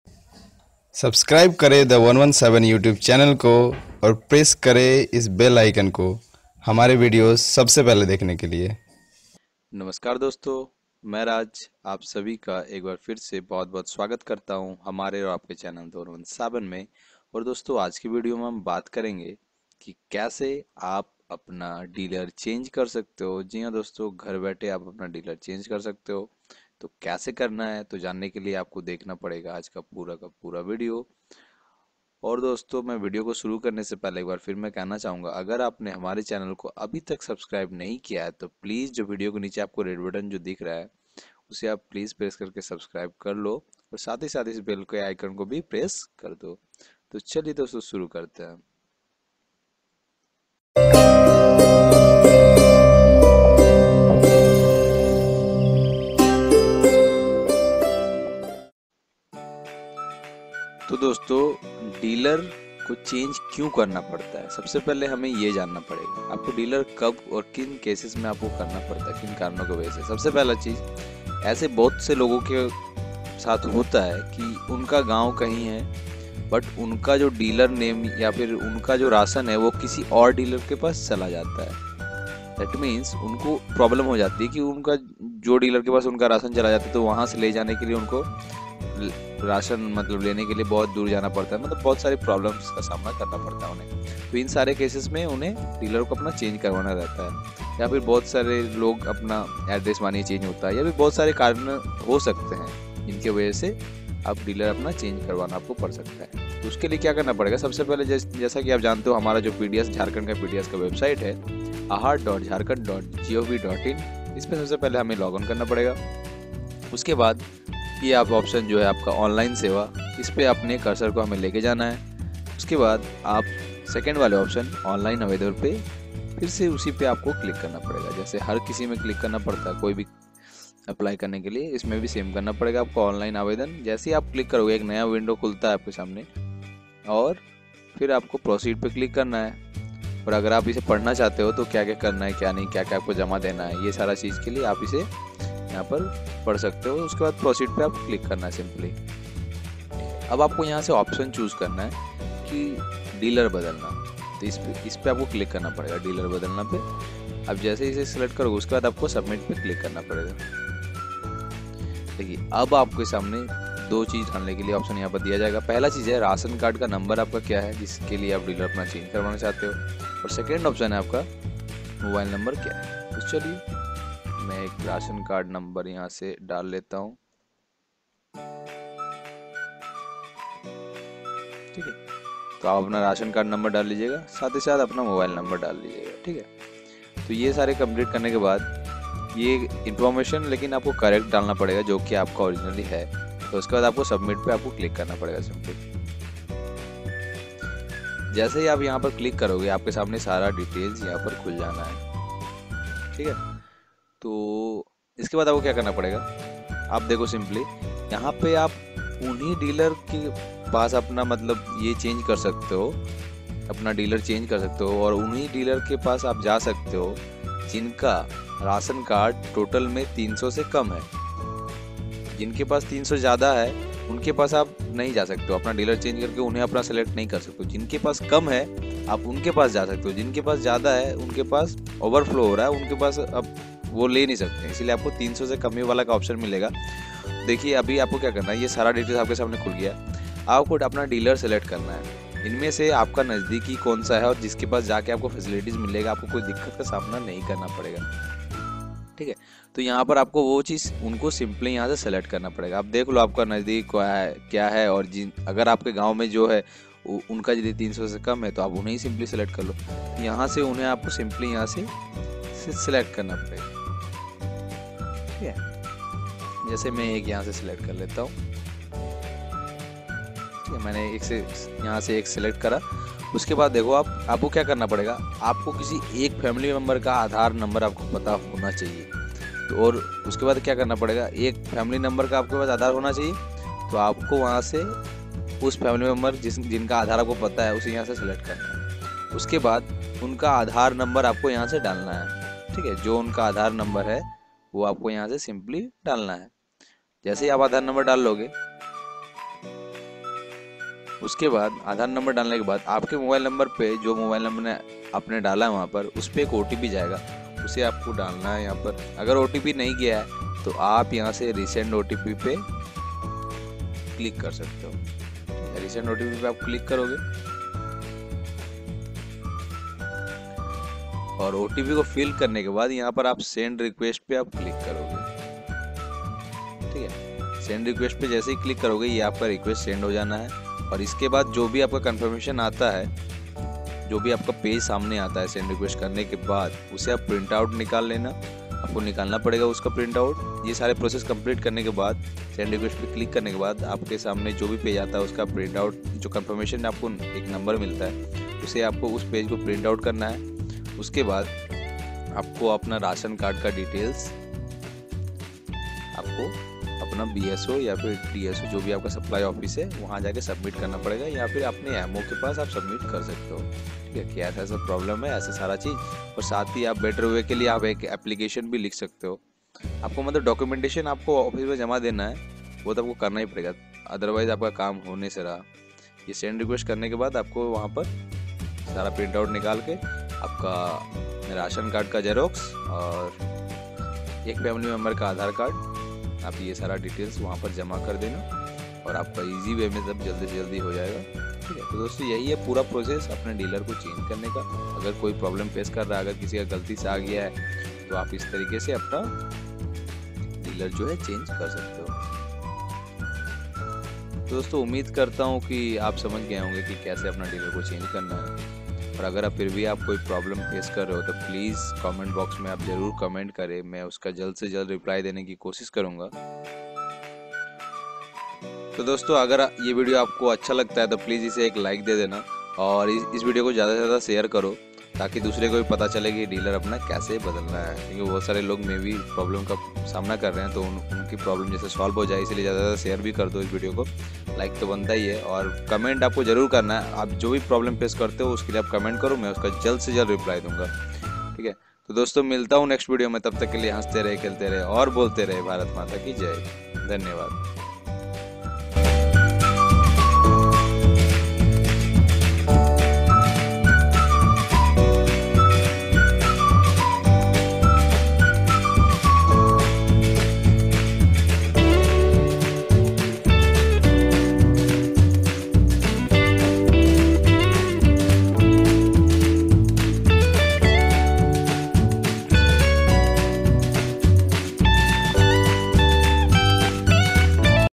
अपना डीलर कैसे बदले | How to change Dealer in Jharkhand PDS Ration Card [The 117]
Namaskar!!!

In this video i have discussed about the process to change the dealer of ration card in jharkhand pds.

This is not the process to remove family member from ration card in jharkhand.

Documents required :-1. Voter id/any address proof
2.acknowledement receipt print out
3.Ration card xerox

Download lost or damaged ration card without card number :-

Change head of the family of ration card in jharkhand :-

The 117 the youtuber of jharkhand